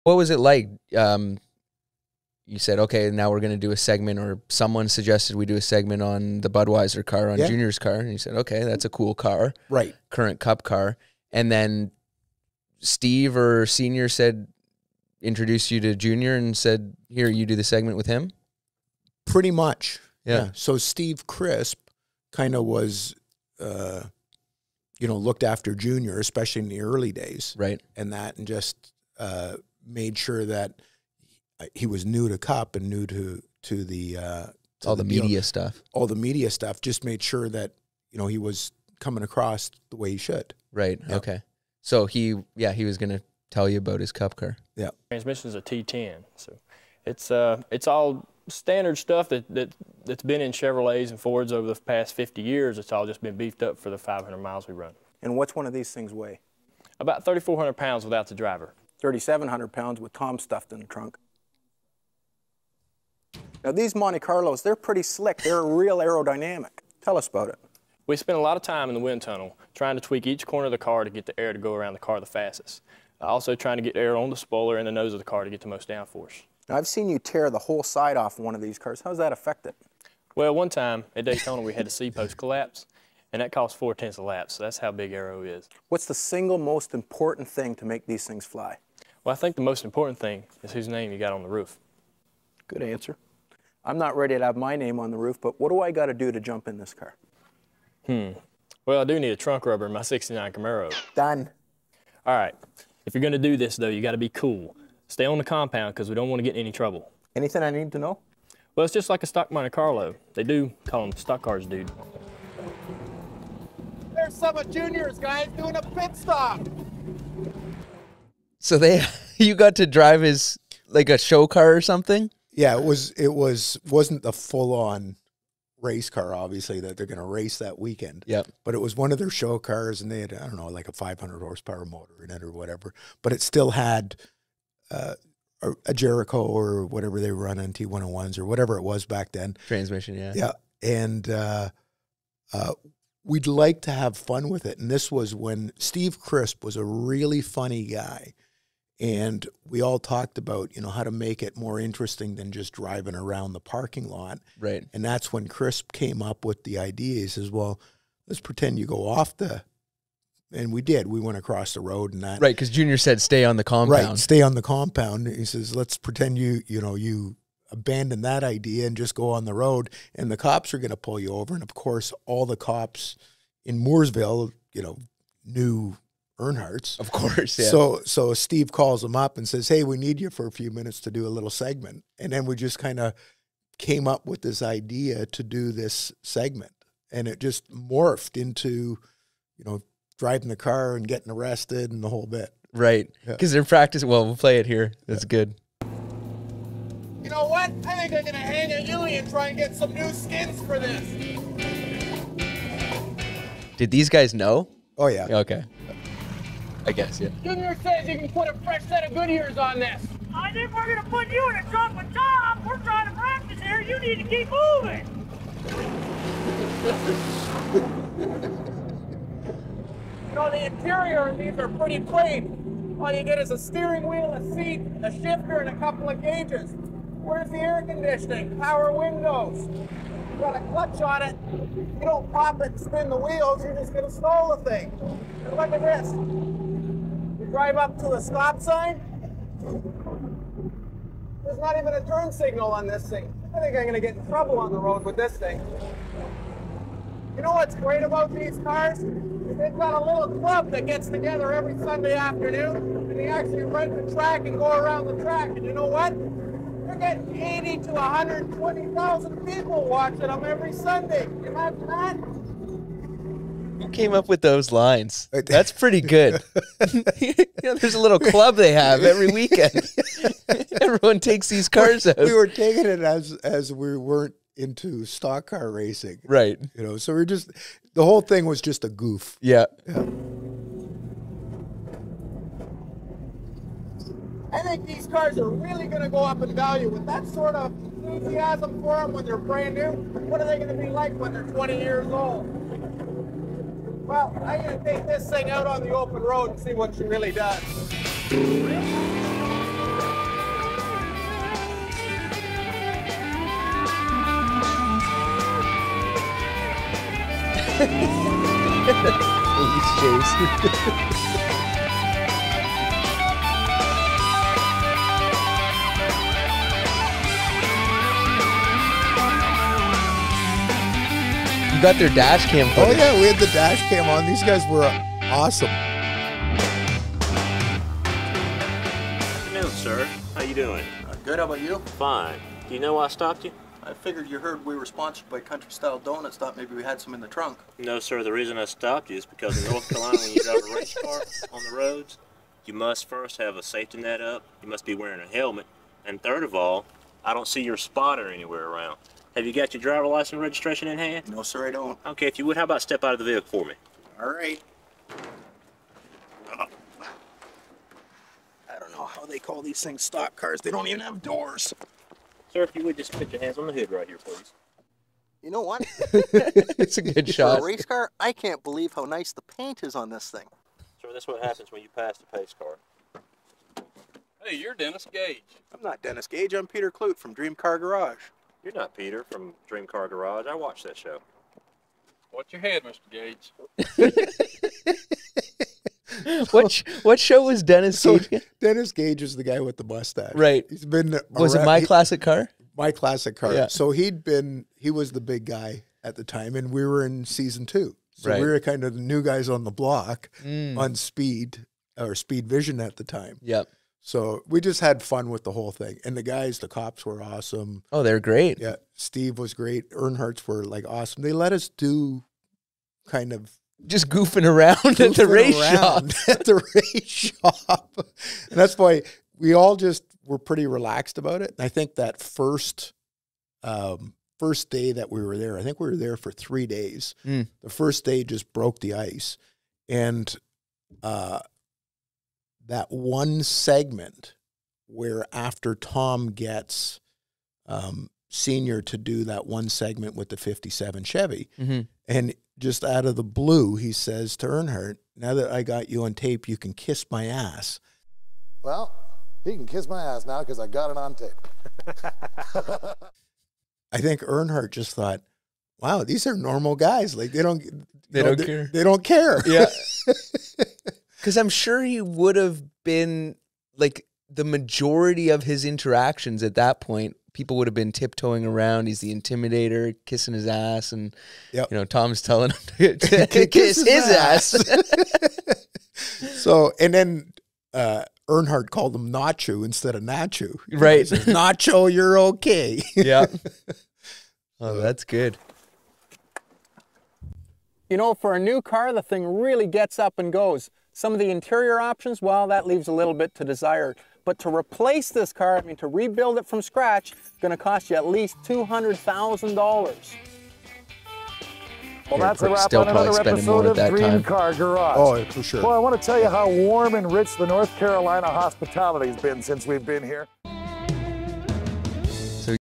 Peter Klutt Gets Arrested! - Dream Car Garage
Peter Klutt got arrested while filming for the show Dream Car Garage. Peter Klutt, the founder of Legendary Motor Car discusses an old DCG episode with his son Gary Klutt on The Gary Klutt Podcast!